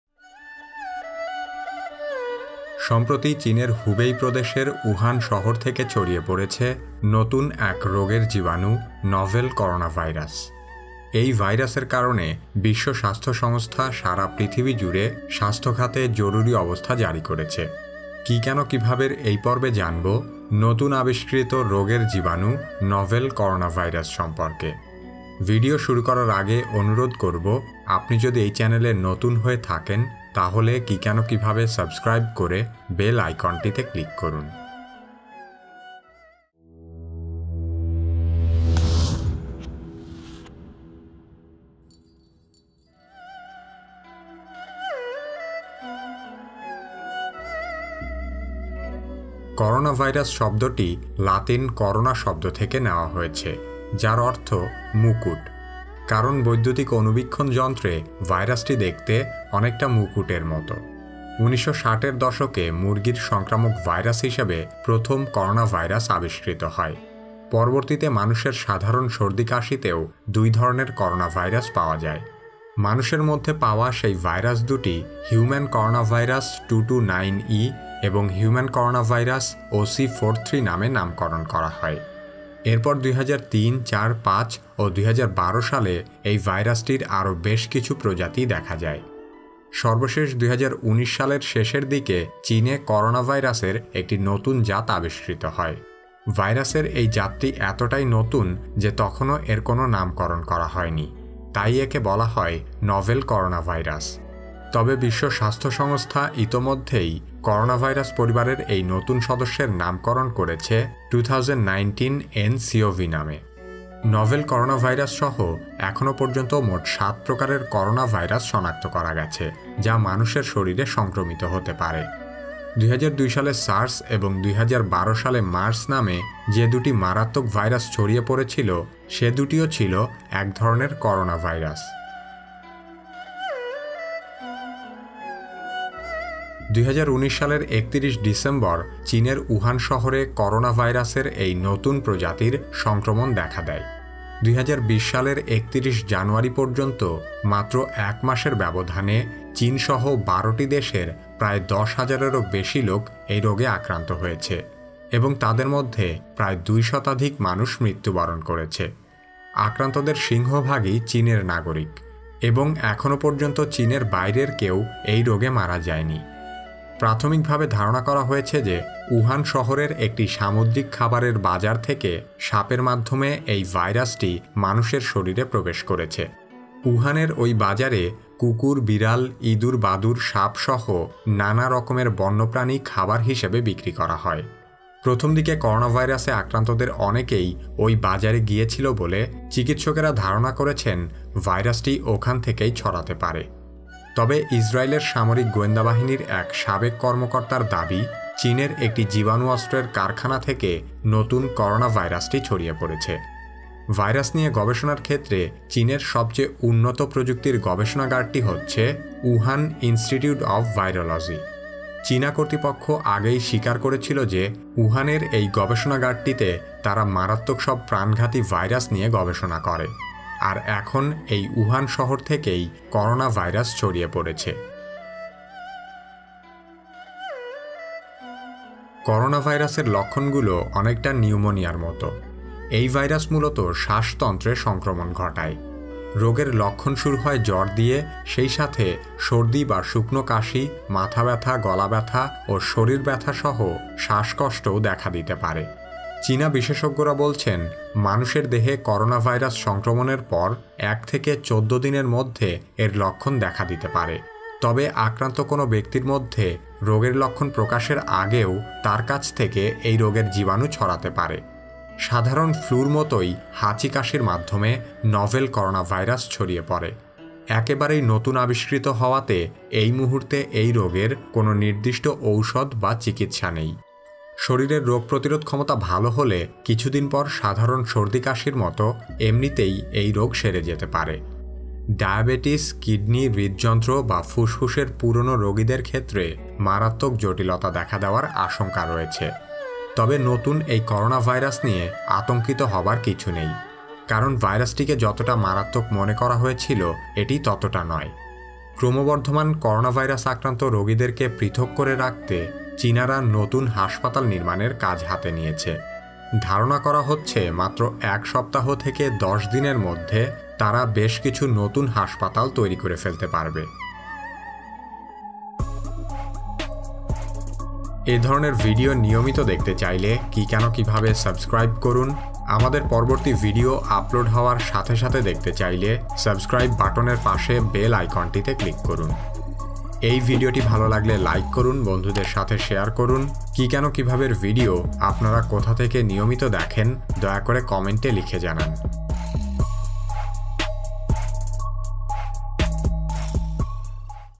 করোনাভাইরাস | কি কেন কিভাবে | Coronavirus | Ki Keno Kivabe
সম্প্রতি চীনের হুবেই প্রদেশের উহান শহর থেকে ছড়িয়ে পড়েছে নতুন এক রোগের জীবানু নভেল করনোভাইরাস। এই ভাইরাসের কারণে বিশ্ব স্বাস্থ্য সংস্থা সারা পৃথিবী জুড়ে স্বাস্থ্য খাতে জরুরী অবস্থা জারি করছে ।

কিকেনকিভাব র এই পর্বে জানব নতুন আবিষ্কৃত রোগের জীবাণু নভেল করোনভাইরাস সম্পর্কে।